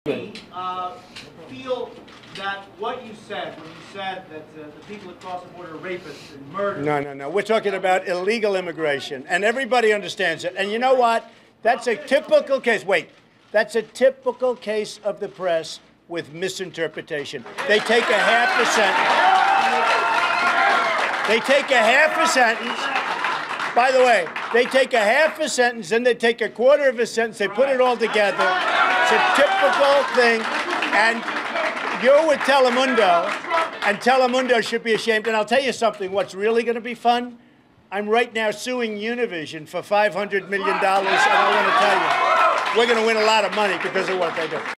Trump Slams Telemundo Reporter at Campaign Stop
Donald Trump criticized a reporter from the Spanish-language channel 'Telemundo' for a question on immigration while speaking at a press confernece in Texas. (July 23)